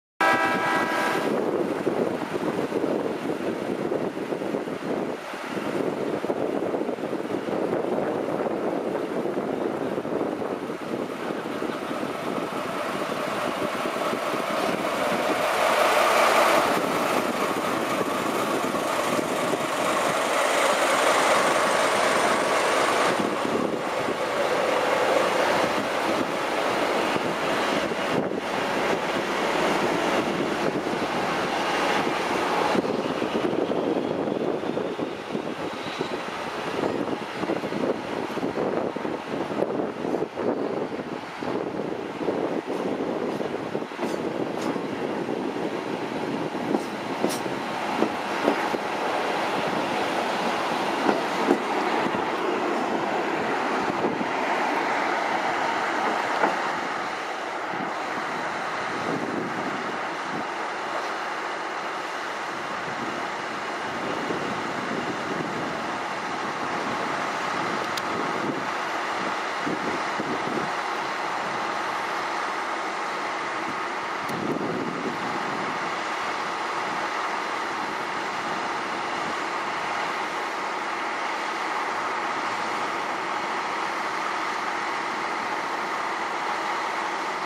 Furano Biei Norokko Train (2)
The Furano Biei Norokko Train departing from Furano Station on the Furano Line.
(recorded July 29th 2014, with Panasonic TZ-7)